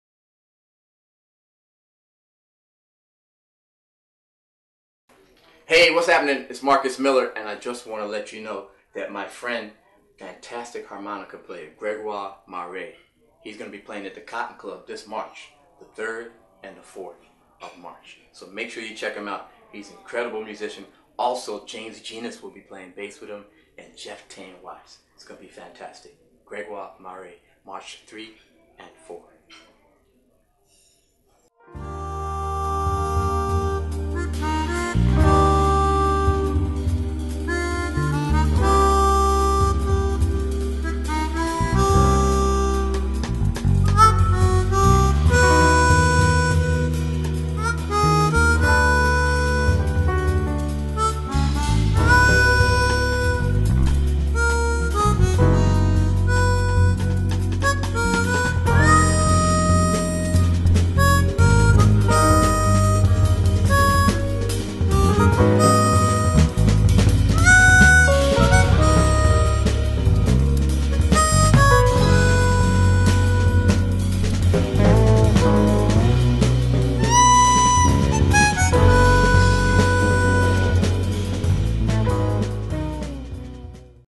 [Message+Trailer] GRÉGOIRE MARET QUARTET : COTTON CLUB JAPAN 2015 trailer
★アーティストからメッセージが届きました。★
To GRÉGOIRE MARET QUARTET From MARCUS MILLER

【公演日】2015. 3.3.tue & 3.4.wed

パット・メセニーとの共演で知られるハーモニカの名手
凄腕メンバーを率いて待望の初リーダー公演

GRÉGOIRE MARET QUARTET
with JOHN BEASLEY, JAMES GENUS & JEFF "TAIN" WATTS
グレゴア・マレ・カルテット・
ウィズ・ジョン・ビーズリー, ジェームス・ジナス & ジェフ・テイン・ワッツ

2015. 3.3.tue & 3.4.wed

MEMBER
Grégoire Maret (hca), John Beasley (p), James Genus (b), Jeff "Tain" Watts (ds)

MUSIC CHARGE
￥7,800